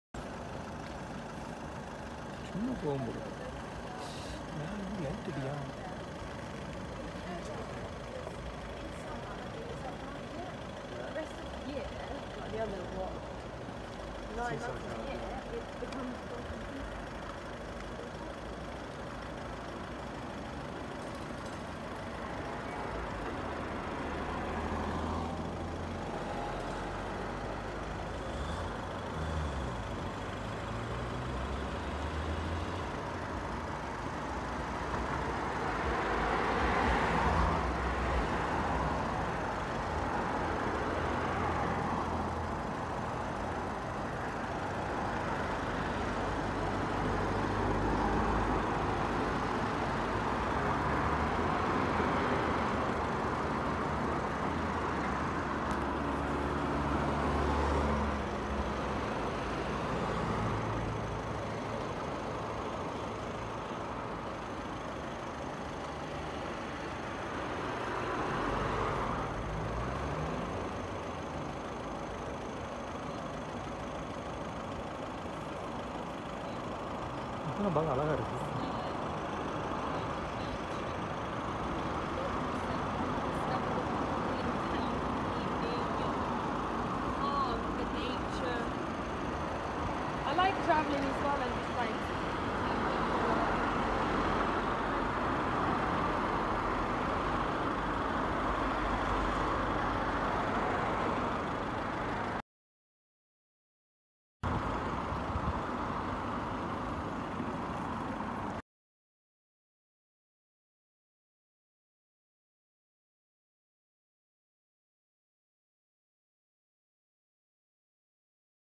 British Airways i360 Brighton by night || Night view of the British Airways i360 uk
Night view of the British Airways i360 it is the worlds tallest moving observation tower on the seafront in Brighton, UK.